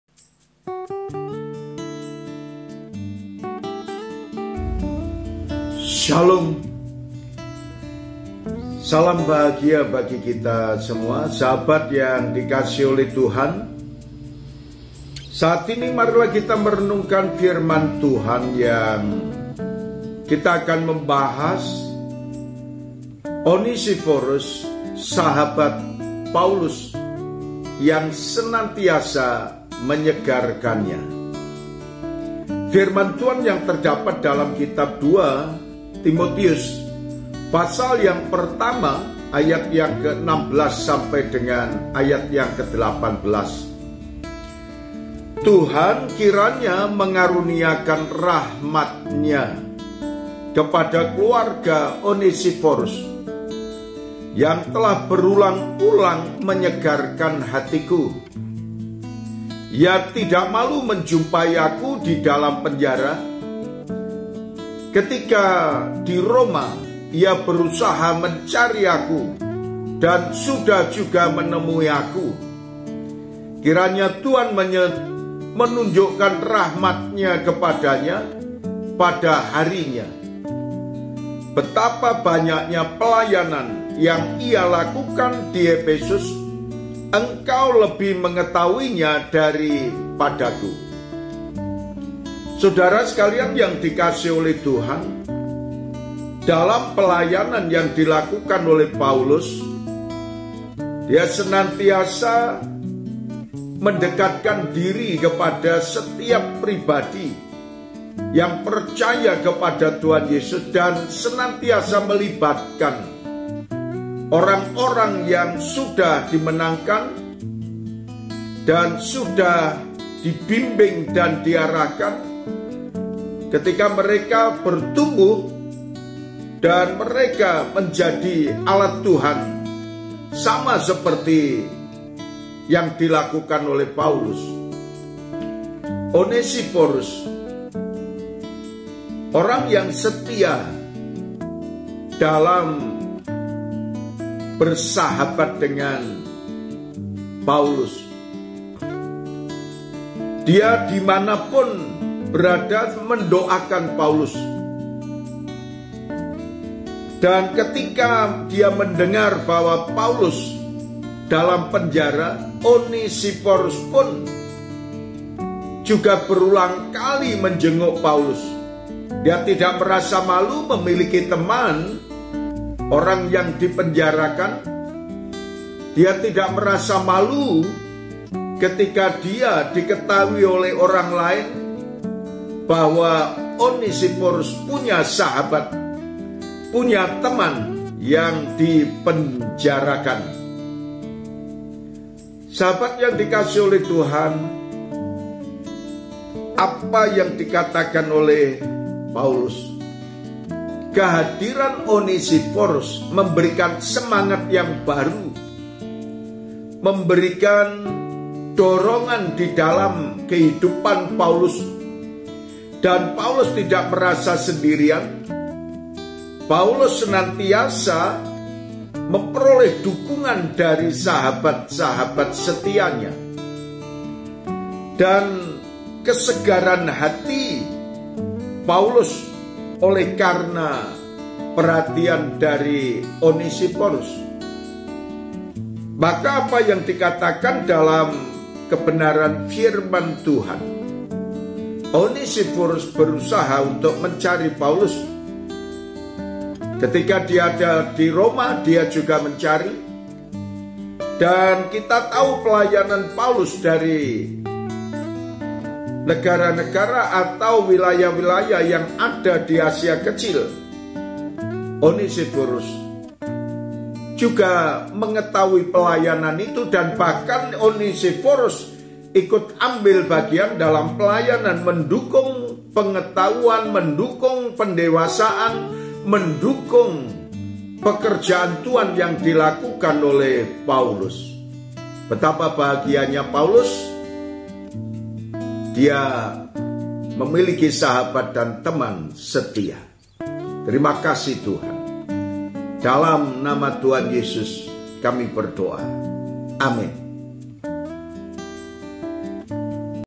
Onesiforus sahabat Paulus yang senantiasa menyegarkannya
Berkat Narwastu
Siraman Rohani Kristiani
Edisi, 6 Februari 2023
Firman: Pdt Timotius Kabul